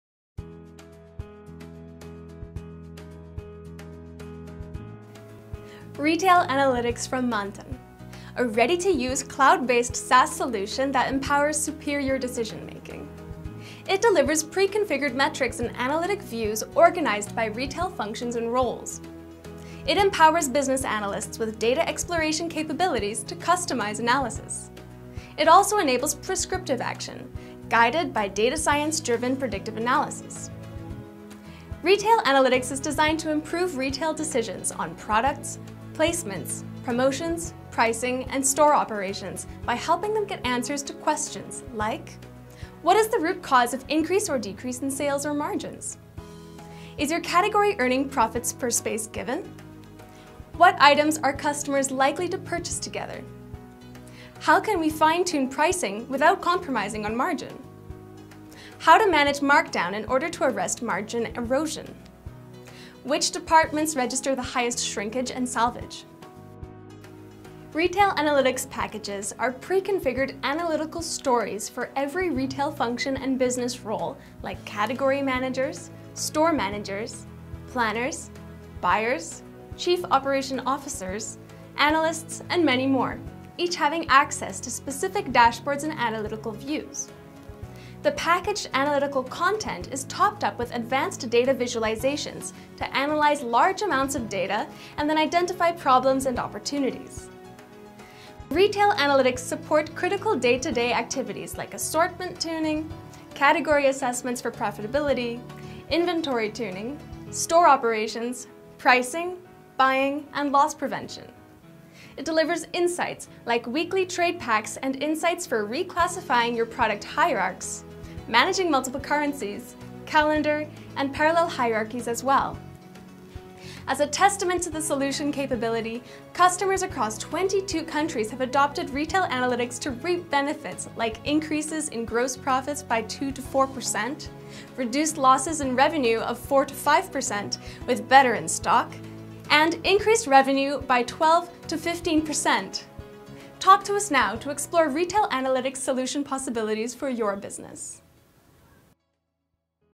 Manthan Retail Analytics Solution Overview
"A comprehensive analytical solution addressing the merchandise and in-store analytical needs of retailers. Spans the analytical spectrum of Discovery, Descriptive, Predictive and Prescriptive analytics.